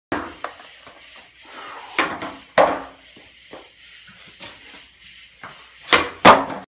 Nightmare Flip
Here I was trying a kick flip and I did a Nightmare Flip